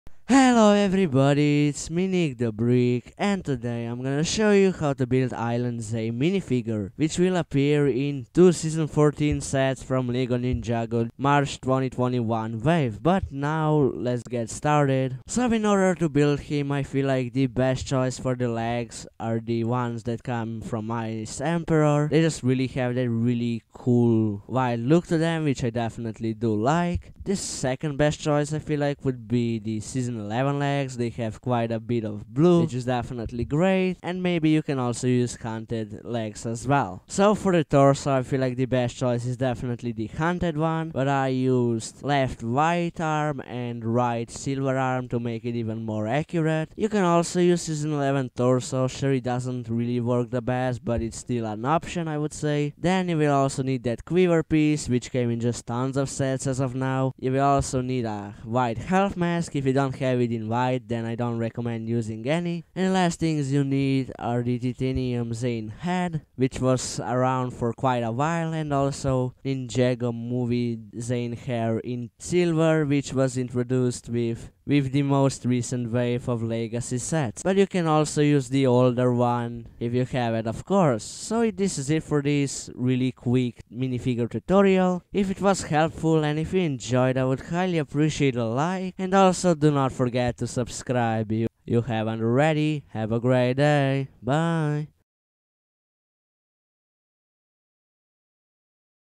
LEGO NINJAGO ISLAND ZANE CUSTOM MINIFIGURE TUTORIAL
This time I will show you how to make a custom mnifigure of Island Zane, which will appear in 2 Lego Ninjago  2021 season 14 sets. What do you think of this custom minifigure, and you like that I show some alternate pieces to use?